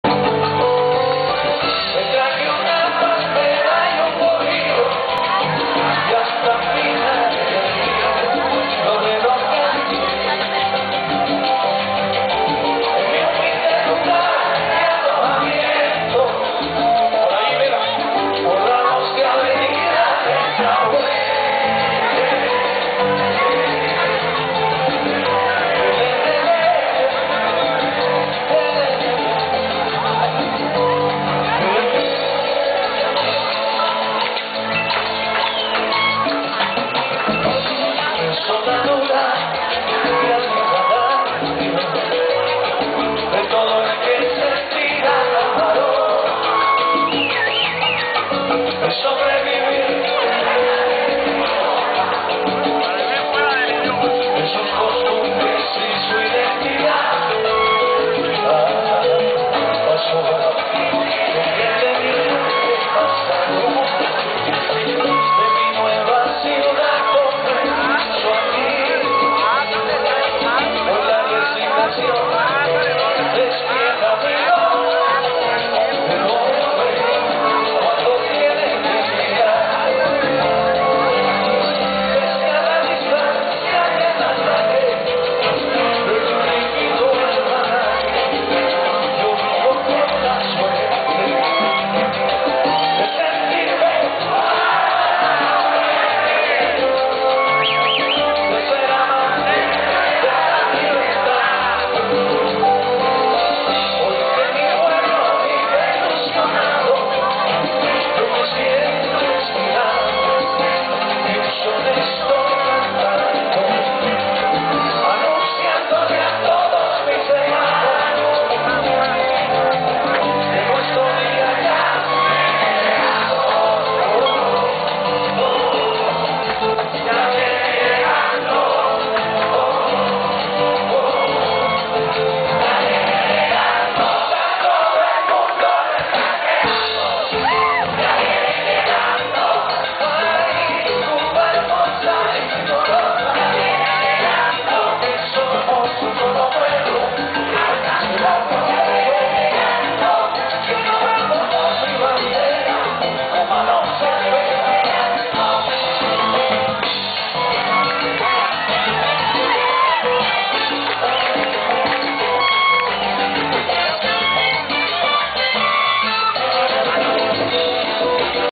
WILLIE CHIRINO
FRAGMENTO DE LA ACTUACION DE WILLY CHIRINO A SU REGRESO AL FESTIVAL DE LA CALLE OCHO EN MIAMI,MI HERMANO MI CUÑADA Y YO GOZANDO DEL CARNAVAL!!! MARZO DEL 2013